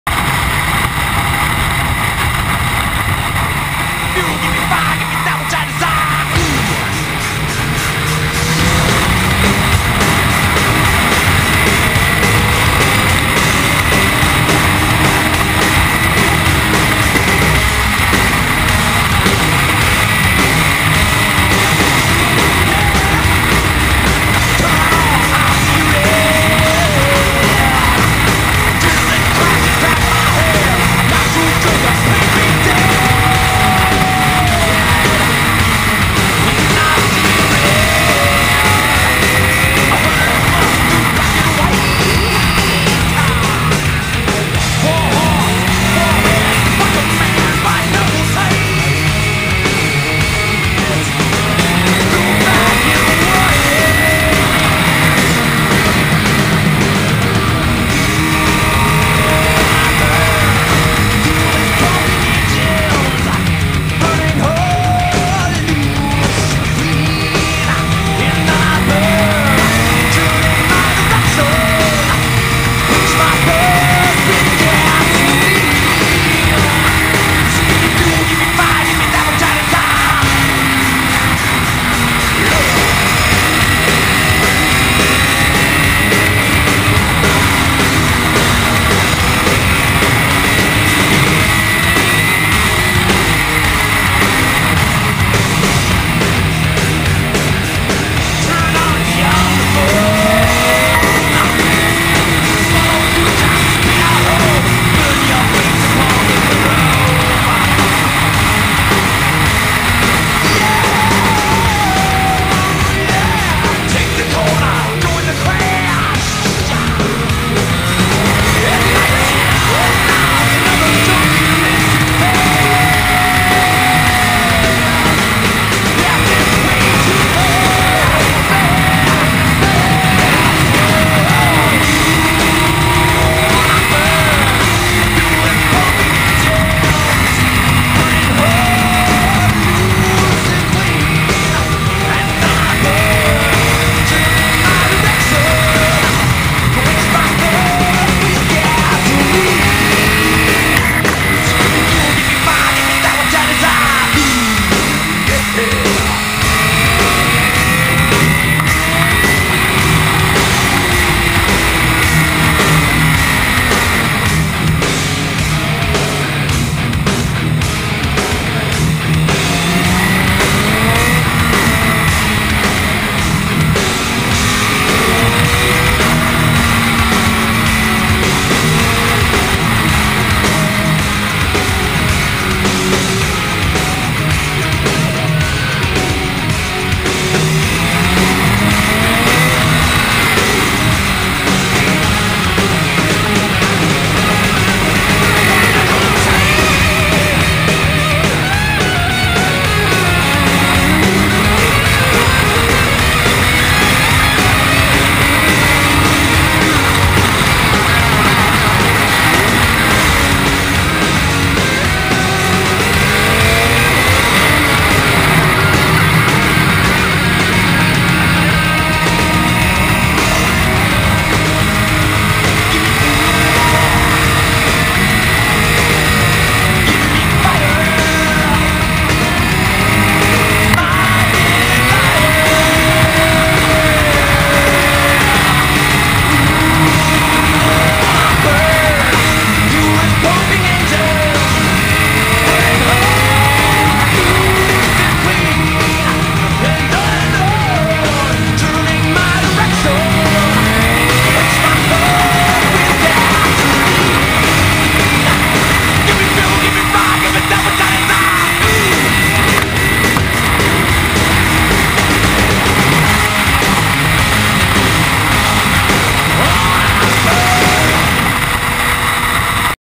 On The Playground: Lotus Elise and CBR 1000RR
On the Playground:

Stock Lotus Elise
CBR 1000RR  - 173 whp